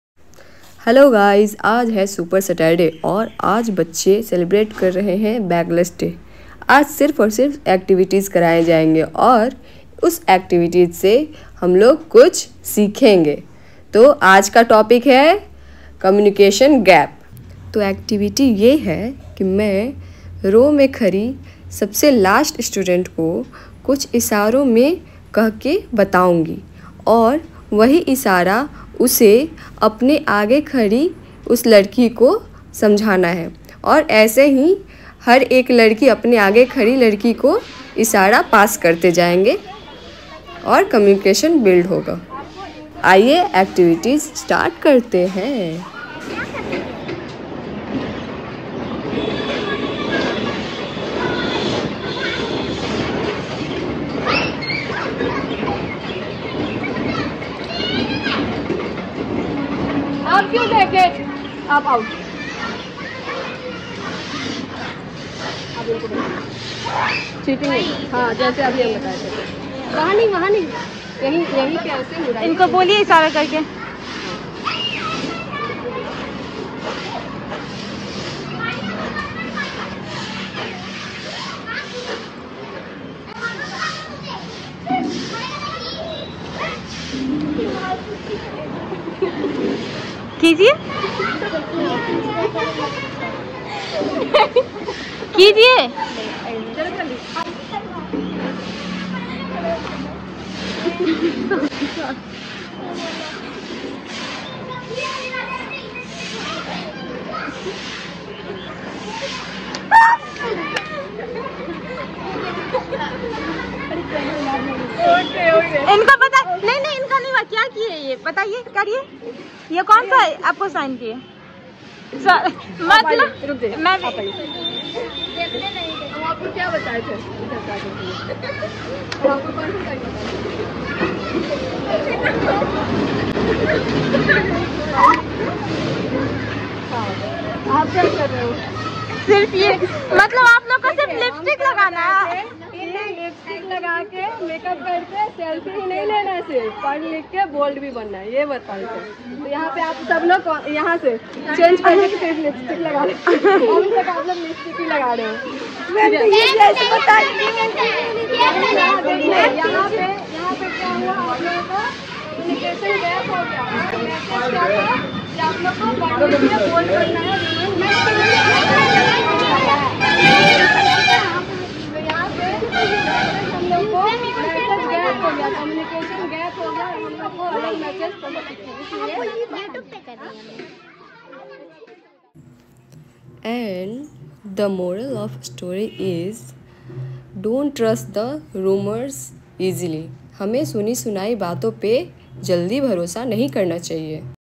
Super Saturday || class activities || communication gap || activities video
The moral ￼of this activities is don’t trust the rumours easily ..￼
School activities, class activities, communication gap activities, children’s fun ￼
government school of Bihar super Saturday activities￼